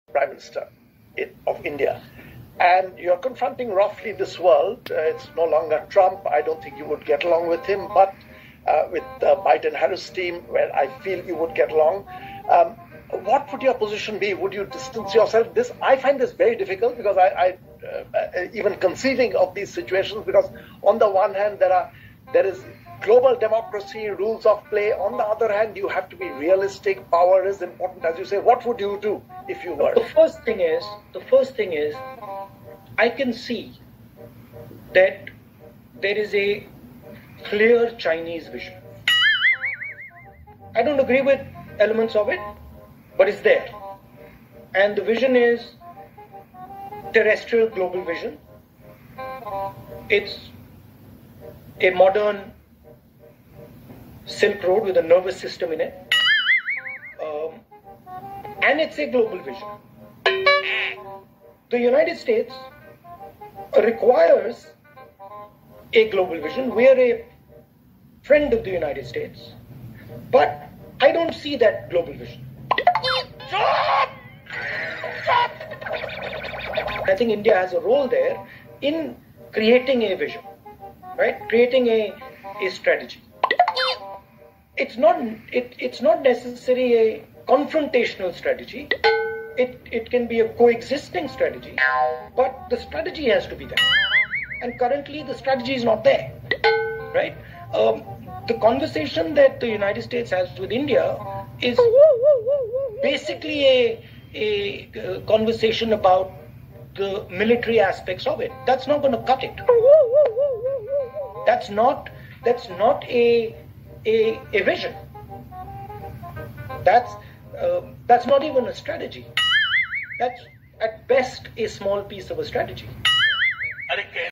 Rahul Gandhi latest 🤯 strategy on china
Shree Shree Shree Rahul Gandhi urfa Pappuendra Maharaj explaining something about strategy which no one is available to understand. What China has to do with it on chinese can tell us if they understand what he is up to tell or lead.